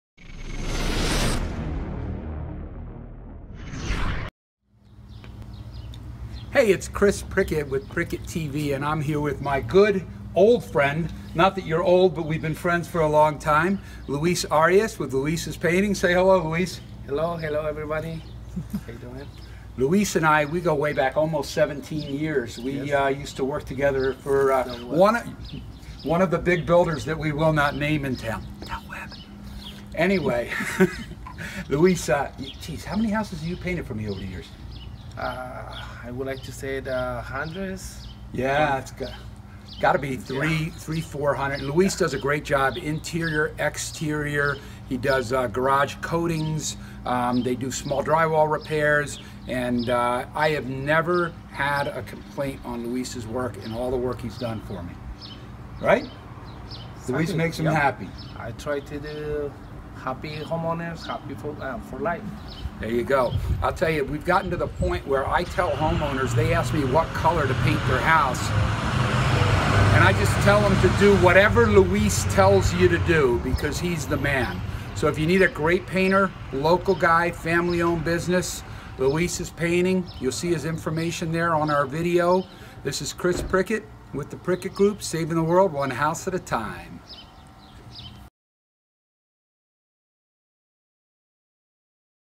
Chris Prickett Interviews Luis' Painting In Anthem, AZ
Chris interviews Luis the Painter...riveting.

Don't forget to check out all of our videos on Prickett.TV